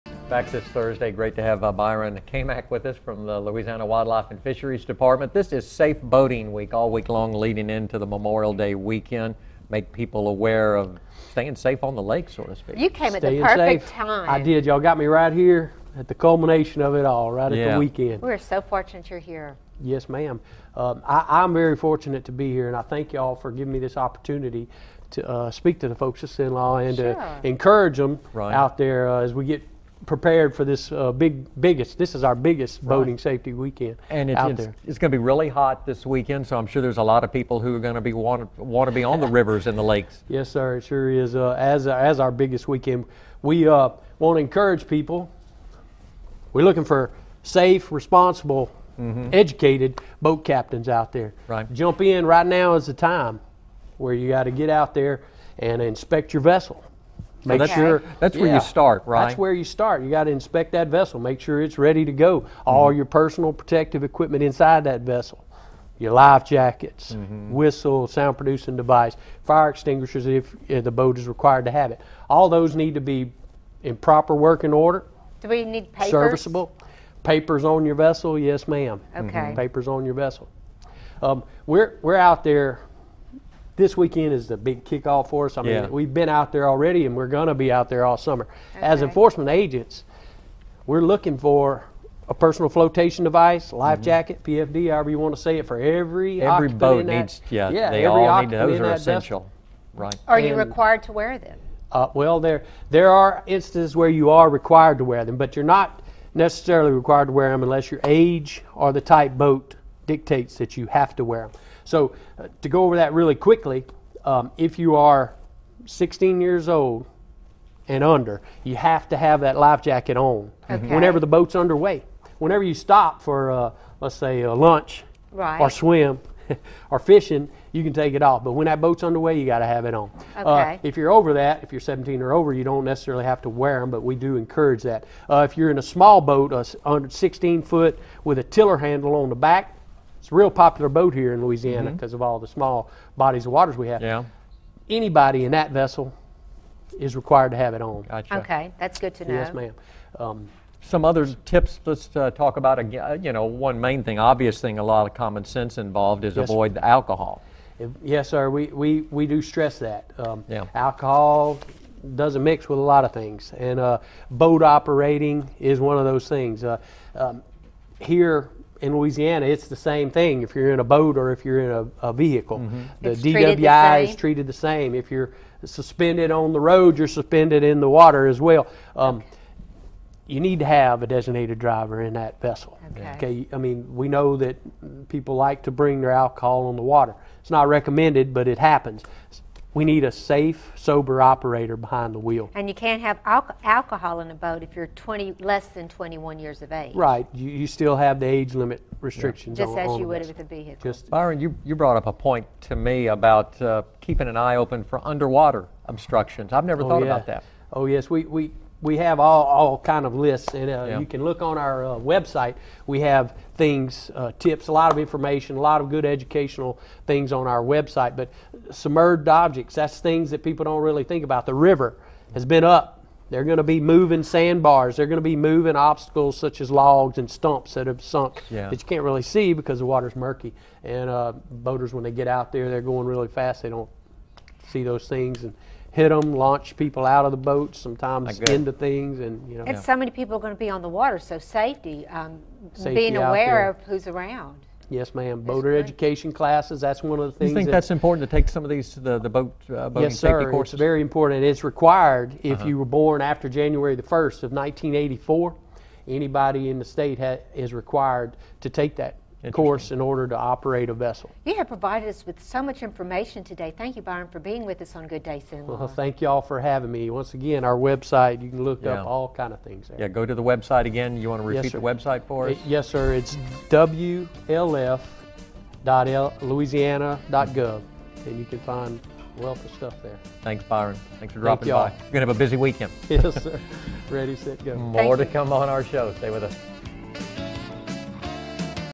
Safe Boating Tips
Byron Cammack joins GDC with some safe boating tips for the summer.